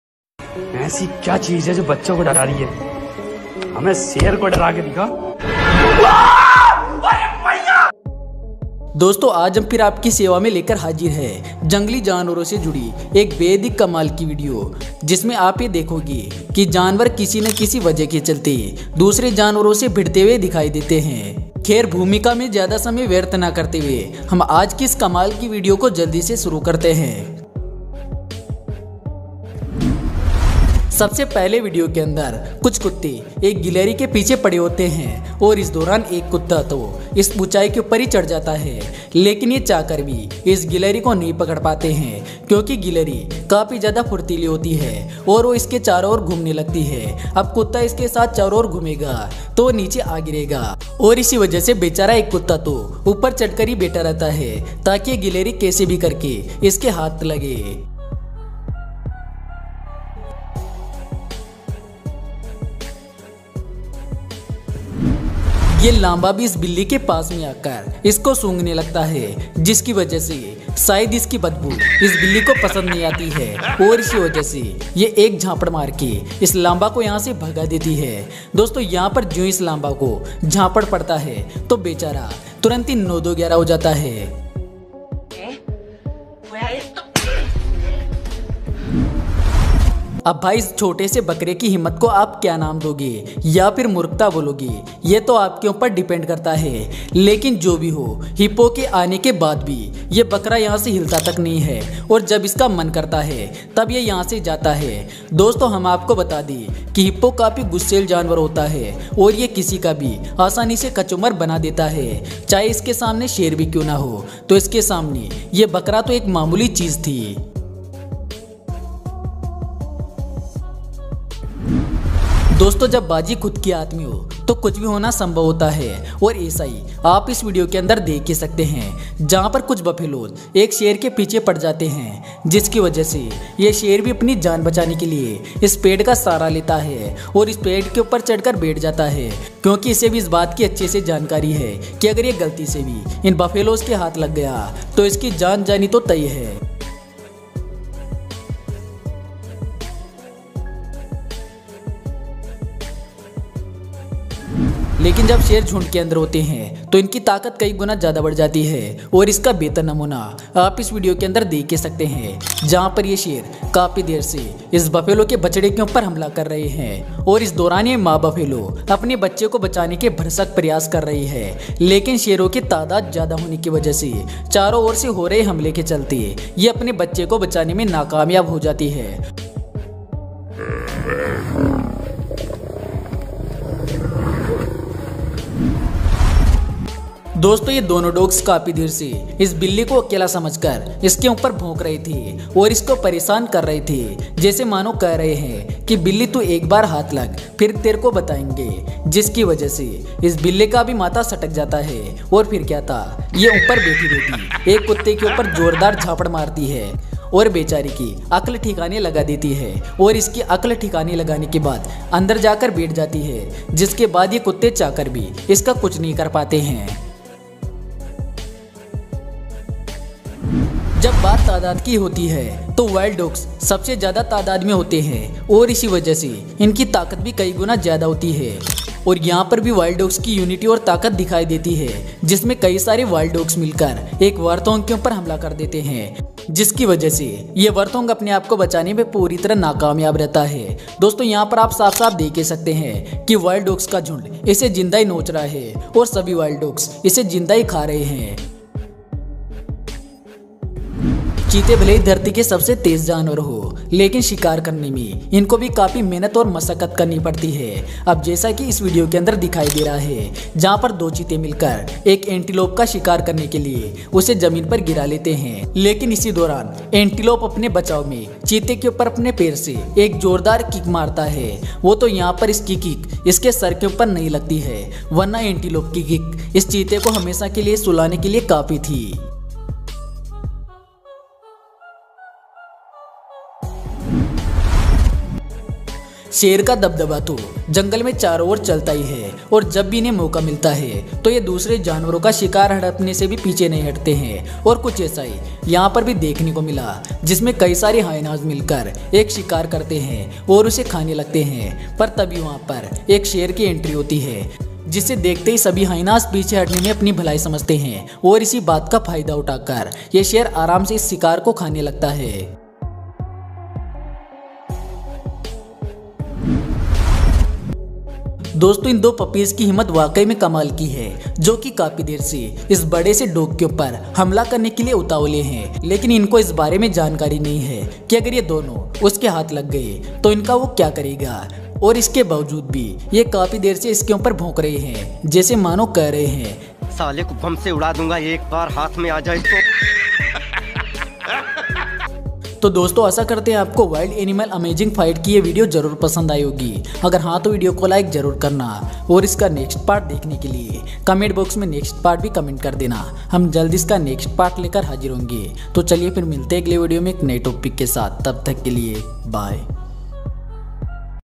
कुत्ता खुद को शेर समझ रहा था बिल्ली ने एक झापड़ सारी हेकड़ी निकाल दी 🤣😂 ll Wild animal amazing fight
thankyou 🙏🙏🙏